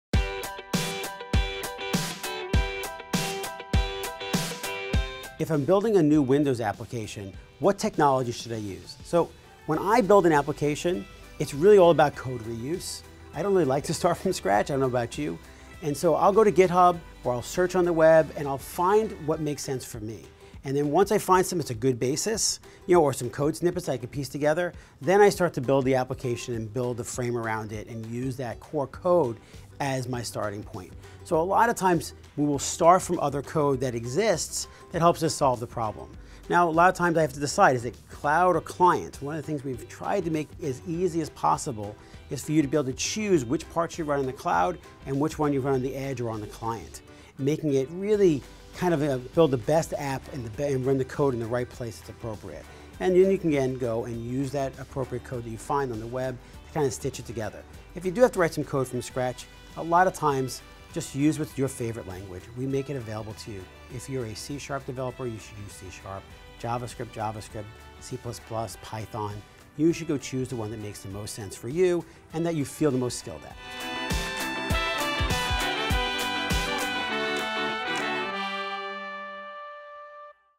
If I'm writing a new Windows client app today, what technology should I use? | One Dev Question
Some great insight and useful tips for building an app today from Kevin Gallo.